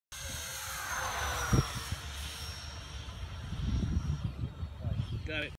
Quick Freewing A-10 Pass twin 64mm 12 blade Changesun fans...
A quick video of my A-10 doing a fast pass at sunset last night.